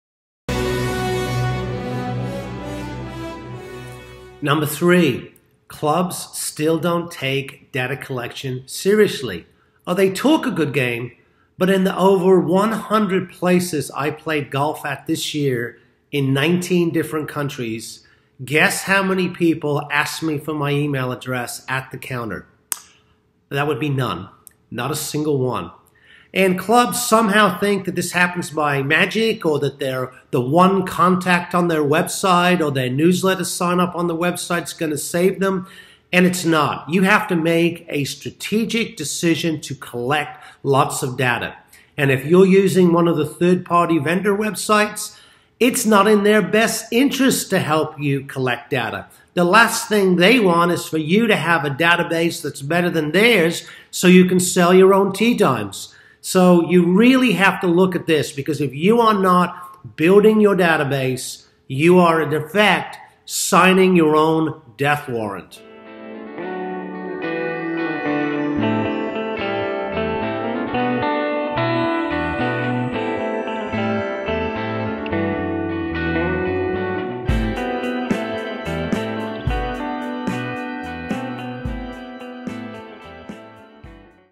The Death of Golf - Reason #3 - No One Takes Data Collection Seriously!
I played over 100 rounds this year all over the world. Guess how many courses asked for my email? That’s would be none!

Websites especially those of third-party tee vendors do a terrible job of collecting data.  It’s not in their best interest to help the course build a list.

While golf courses say they understand the value of data few actually do.